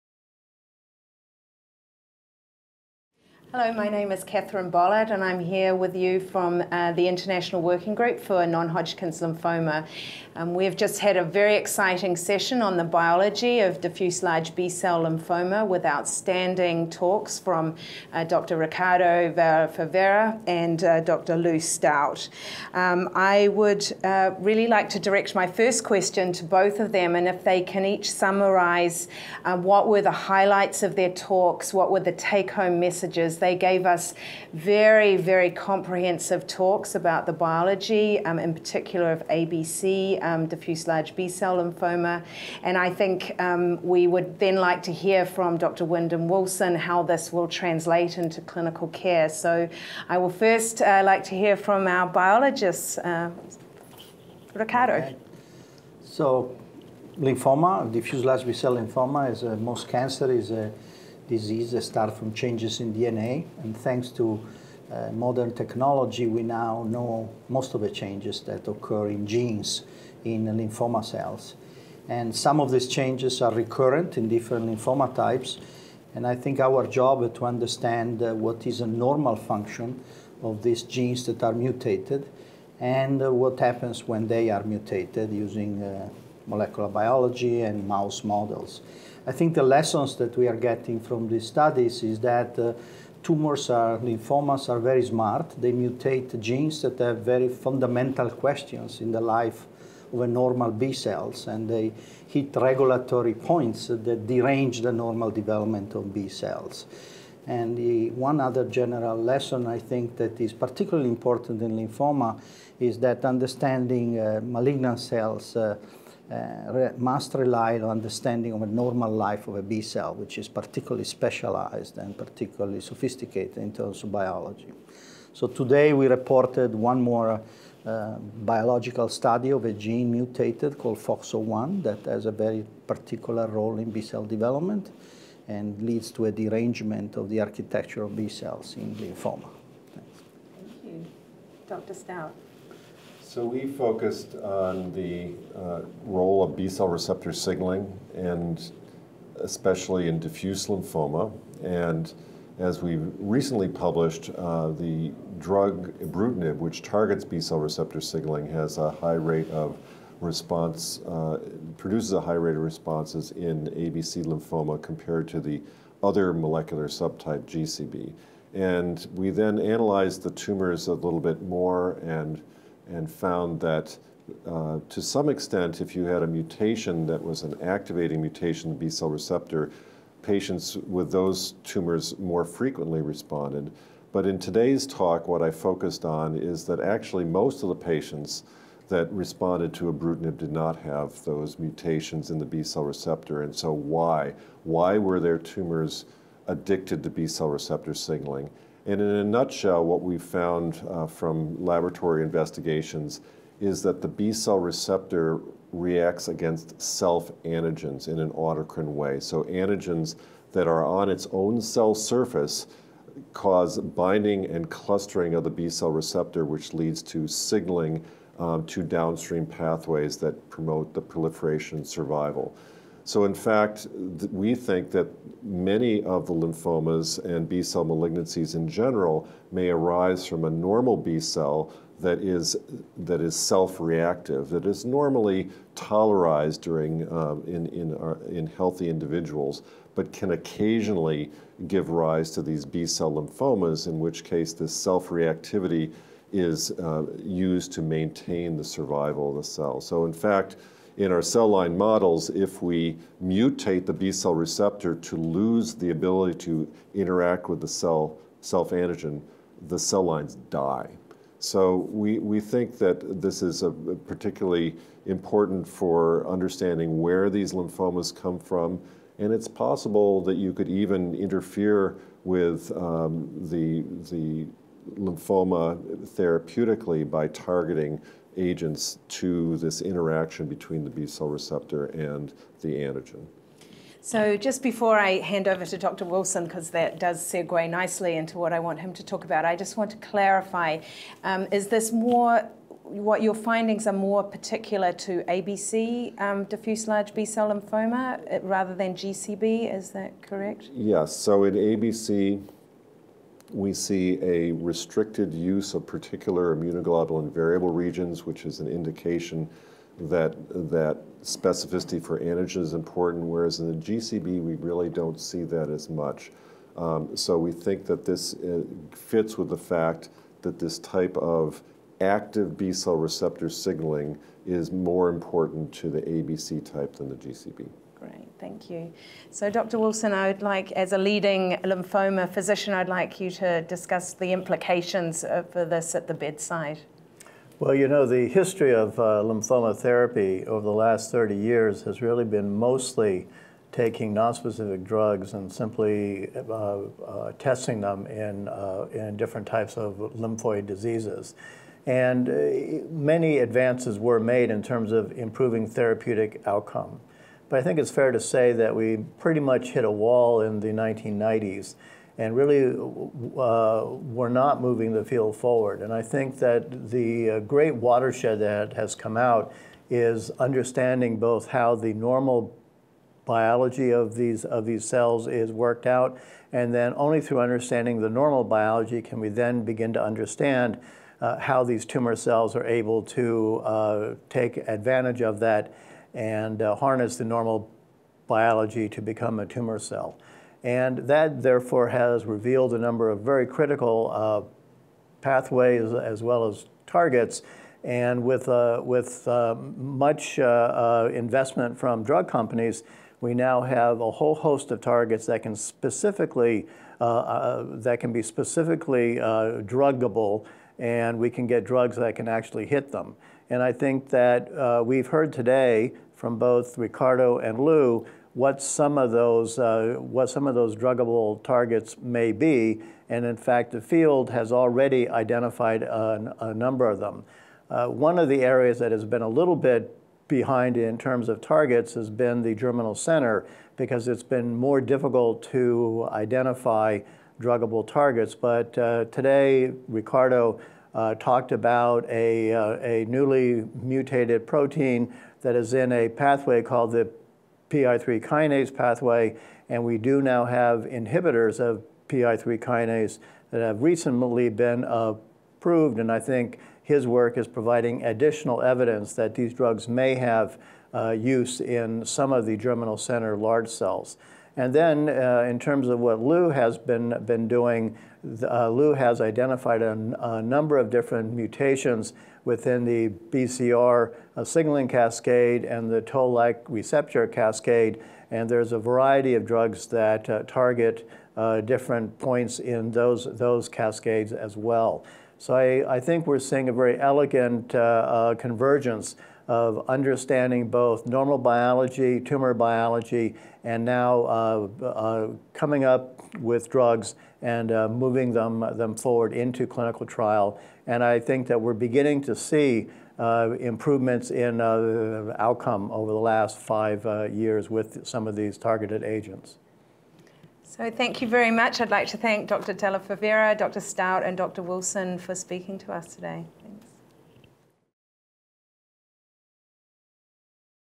iwNHL 2015 roundtable: Biology of diffuse large B-cell lymphoma
At the International Workshop on Non-Hodgkin Lymphoma (iwNHL) 2015 meeting, Catherine M. Bollard, MBChB, MD, FRACP, FRCPA, of Children's National Health System and The George Washington University, Washington, DC, chairs a discussion with Wyndham Wilson, MD, PhD, of the National Cancer Institute, Bethesda, MD, Louis M. Staudt, MD, PhD, of the National Cancer Institute, Bethesda, MD, and Riccardo Dalla-Favera, MD, of the Institute of Cancer Genetics, Columbia University, New York, NY, on the clinical implications of the current understanding of the biology of B cells and diffuse large B-cell lymphoma (DLBCL) subtypes, including activated B-cell-like (ABC) DLBCL and germinal-center B-cell-like (GCB) DLBCL. They explain that these subtypes are characterised by significant differences in outcomes and responses to treatment.